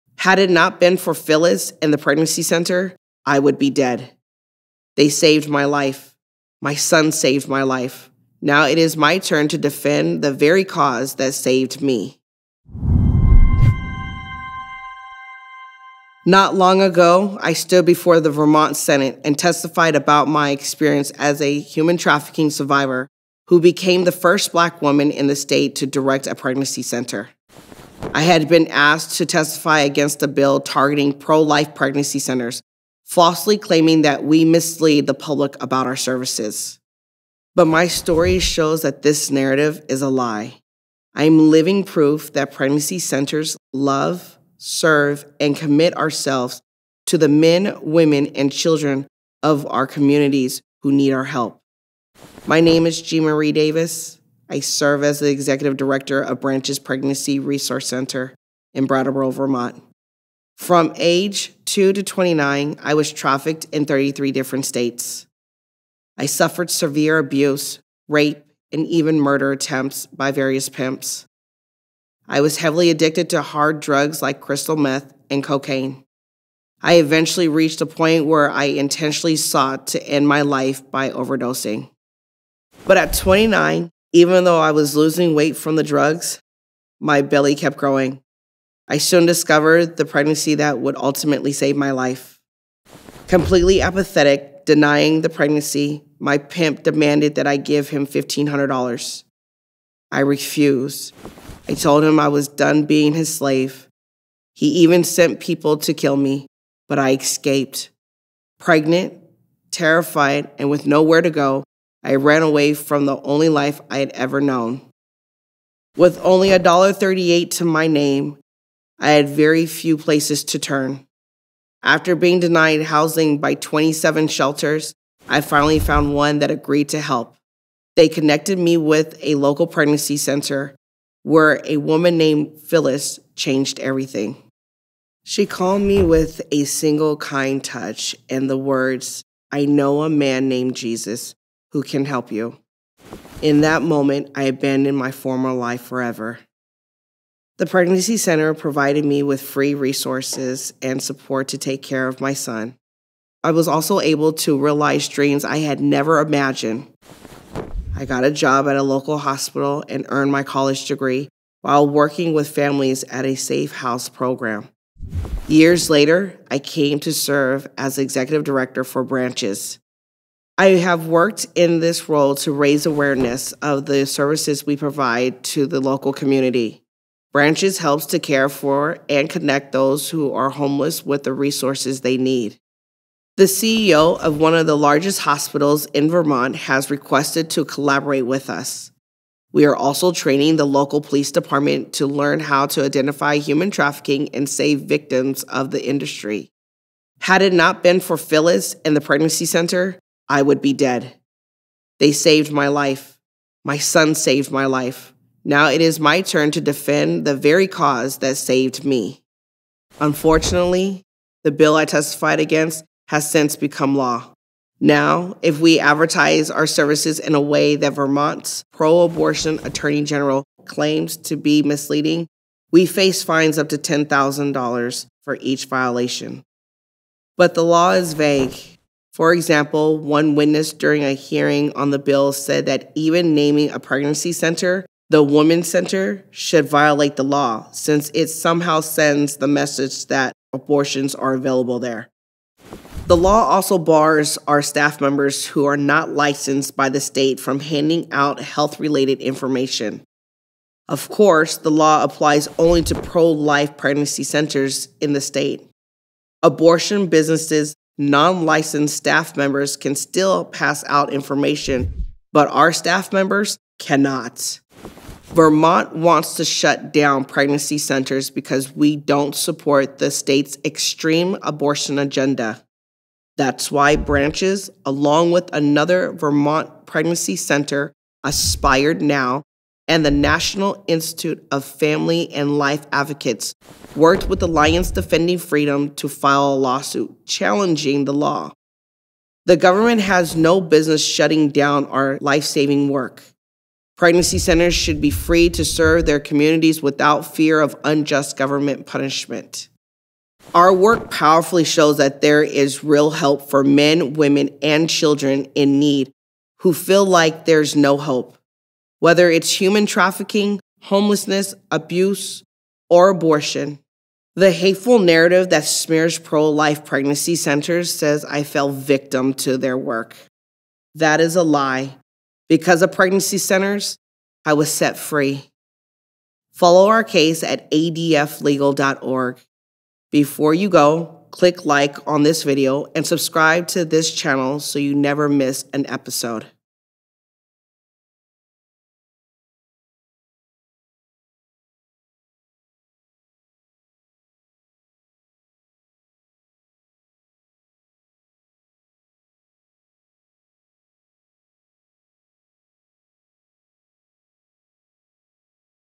She was Rescued from Human Trafficking, Now Jean Marie is Standing for Life Every Day
Not long ago, Jean Marie Davis stood before the Vermont Senate and testified about her experience as a human trafficking survivor who became the first black woman in the state to direct a pregnancy center.

Jean Marie had been asked to testify against a bill targeting pro-life pregnancy centers, falsely claiming that they “mislead” the public about their services.

But Jean Marie’s story shows that this narrative is a lie. She is living proof that pregnancy centers love, serve and commit themselves to the men, women and children of their communities who need their help.

The hateful narrative that smears pro-life pregnancy centers would have some believe that Jean Marie fell “victim” to their work. That is a lie. Because of pregnancy centers, She was set free.